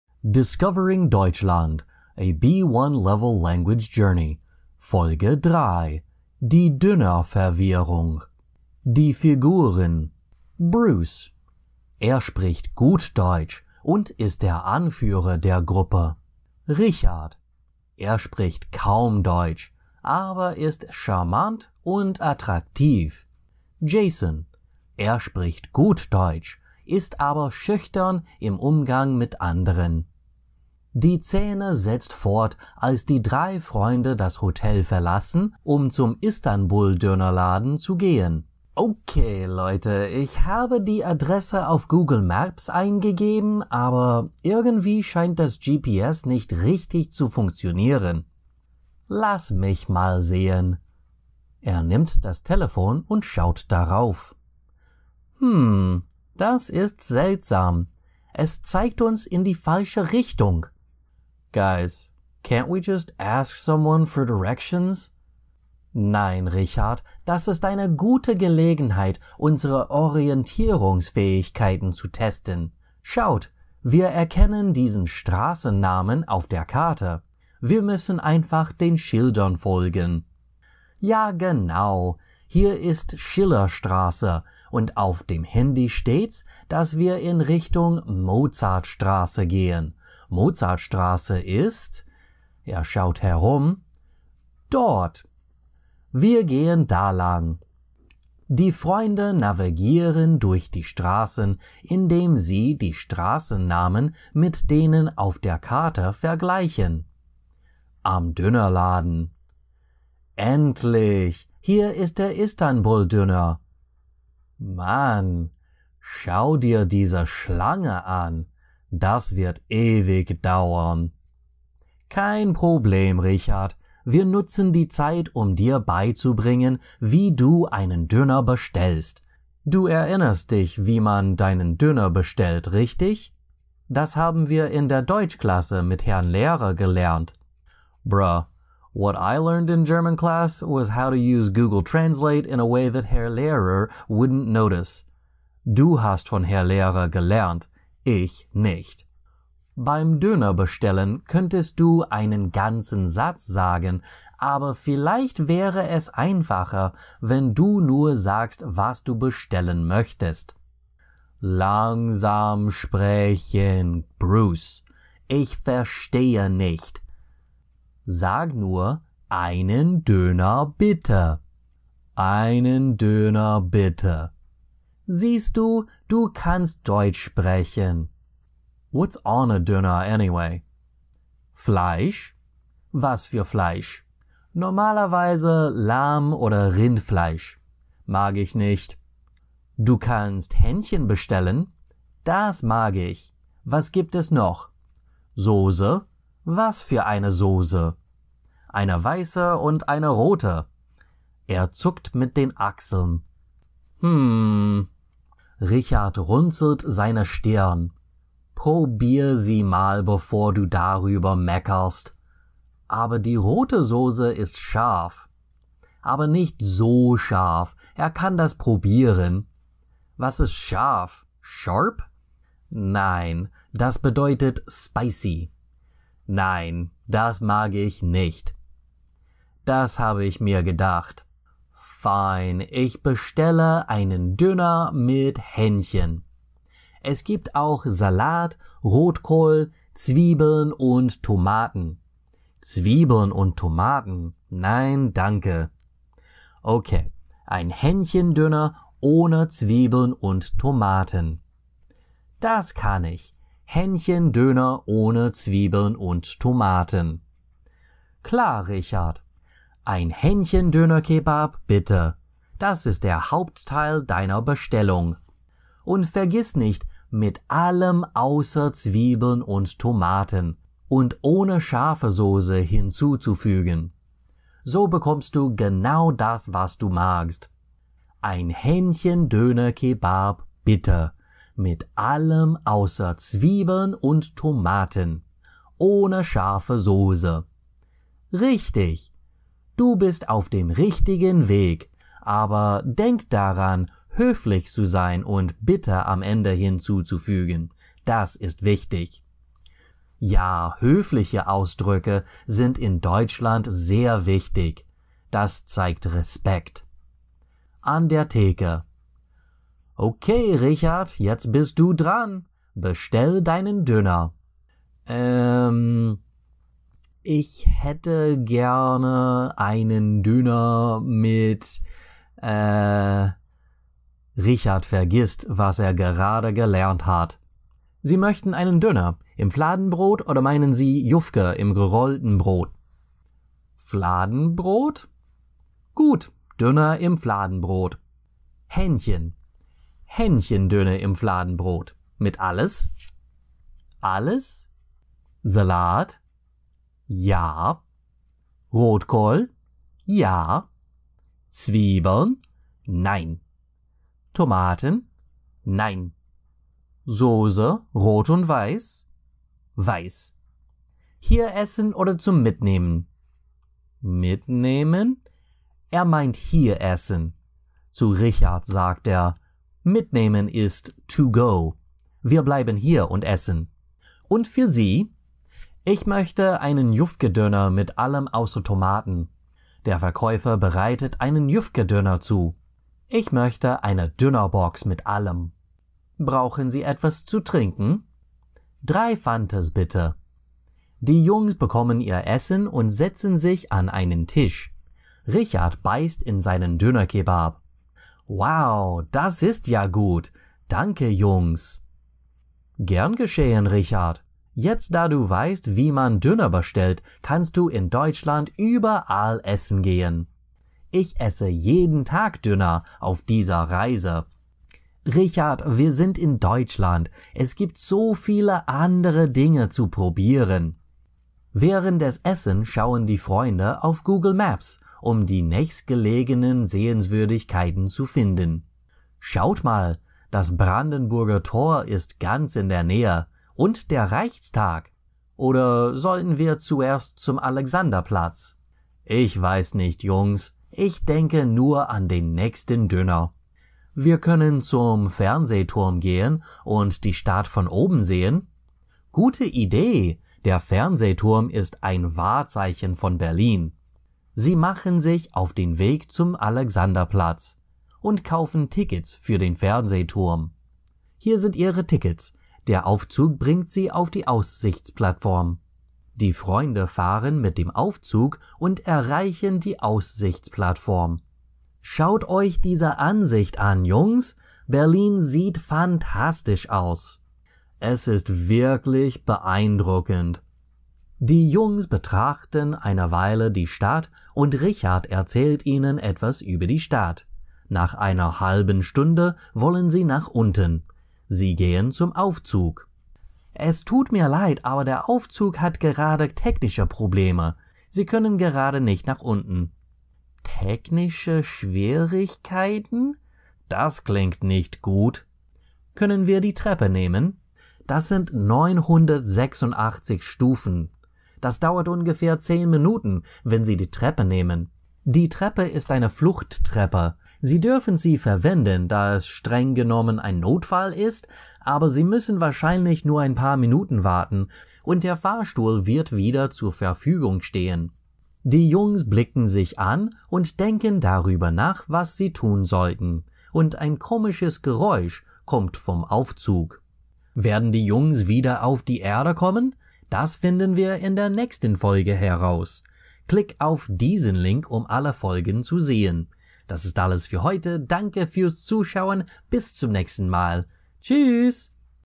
Die Döner-Verwirrung | Discovering Deutschland Ep. 3
Welcome to Episode 3 of Discovering Deutschland: Intermediate B1 German Listening Adventure!

In this episode, our friends Bruce, Richard, and Jason set out for the famed Istanbul Döner, where Bruce and Jason teach Richard how to order food in German. After mastering phrases like “without onions, please,” they embark on a sight-seeing journey through Berlin—with a few unexpected twists along the way.

🥙 Episode Highlights:
- Ordering Food in German: Follow along as Richard learns to navigate German menu options and make special requests like a pro.
- Exploring Berlin Without GPS: Watch the trio explore Berlin the old-fashioned way, testing their navigation and German skills!
- Real-Life Language Practice: Pick up everyday phrases and vocabulary for ordering food, getting around, and handling small misunderstandings.

Perfect for B1-level learners aiming to expand practical vocabulary and gain confidence, Discovering Deutschland offers language skills and laughs in one engaging series. Viel Spaß! 🌟🇩🇪

In this episode:
0:00 Intro
0:30 Szene 1
1:47 Szene 2
5:06 Szene 3
6:30 Szene 4
7:34 Szene 5

❤️ Become a member of Herr Antrim's Deutschlerner Club to get access to extra German learning materials

📖 Herr Antrim's Books on Amazon*